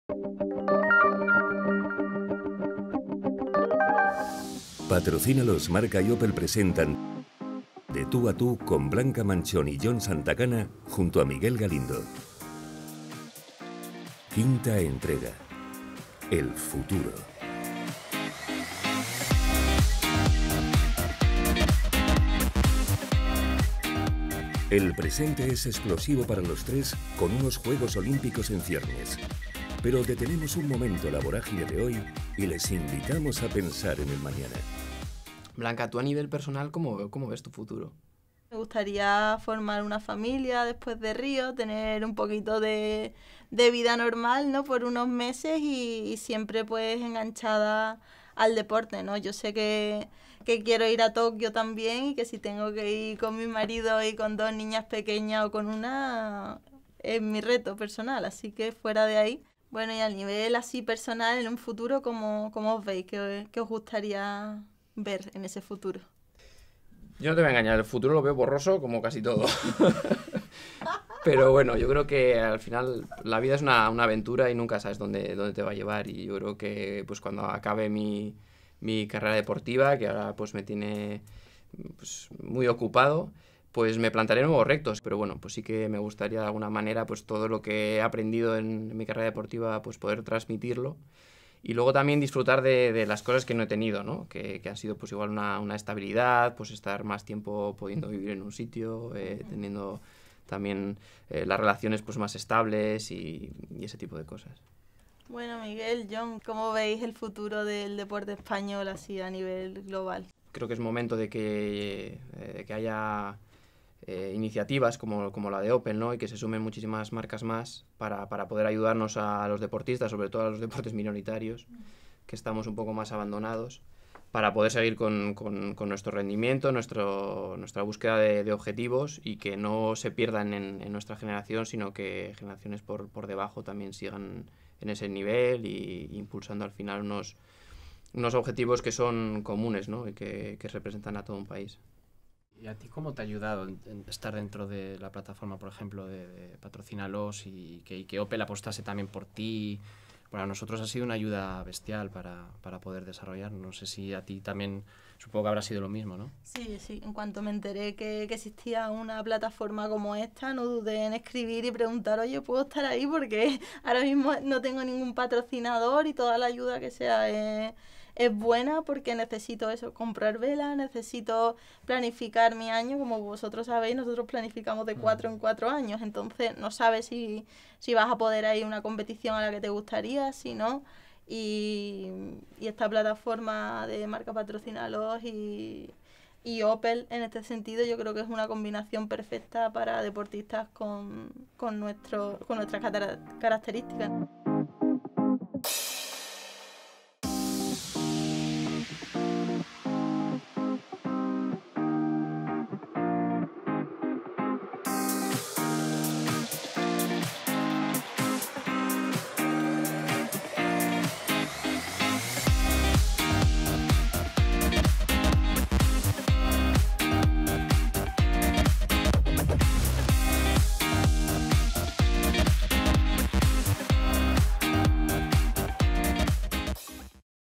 Jon Santacana y Blanca Manchón. El futuro
¿Quieres conocer un poco más a Jon Santacana, esquiador paralímpico con menos de un 5% de visión y a Blanca Manchón, 5 veces campeona del mundo de windsurf? Dale al play y ¡disfruta!